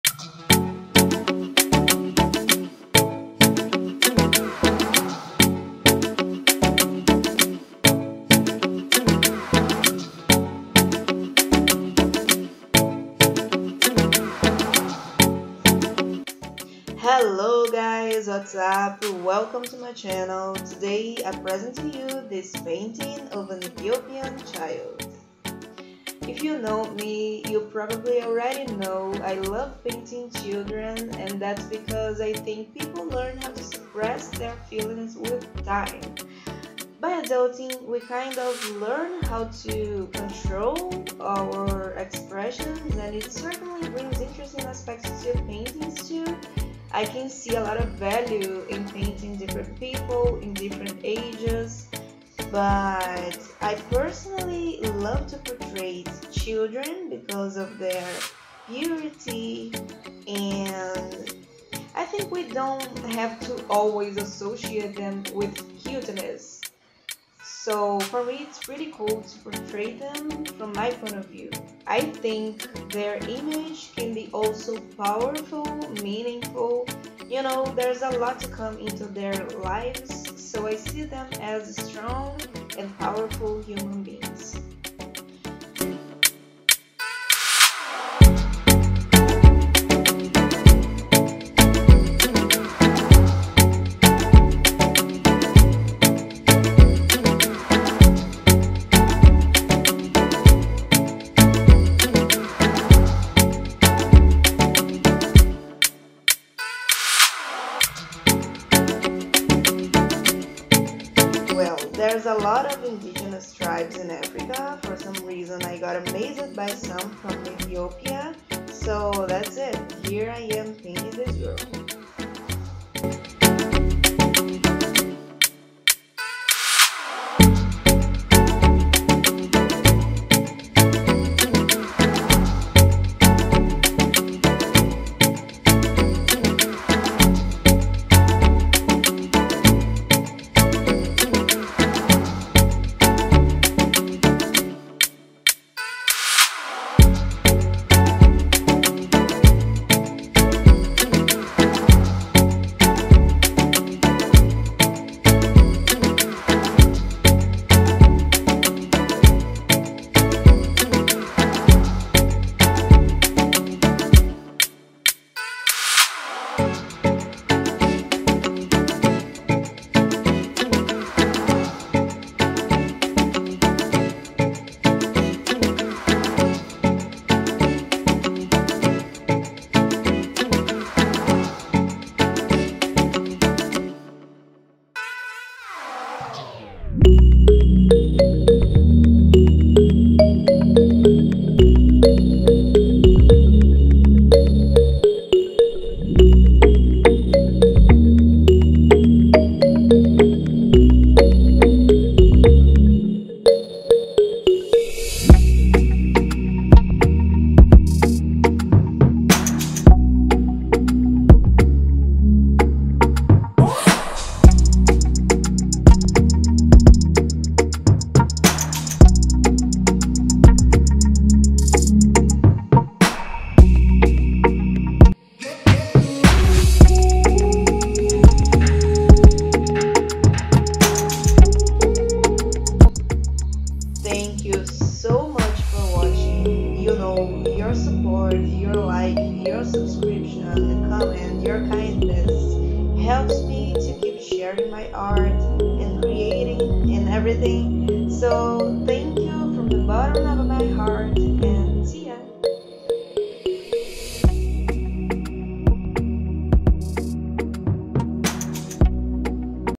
Painting an Ethiopian Child 🌍
Today I present to you this painting of an Ethiopian child. There’s a lot of wonderful indigenous tribes in Africa, for some reason I got amazed by some from Ethiopia. I hope you enjoy it.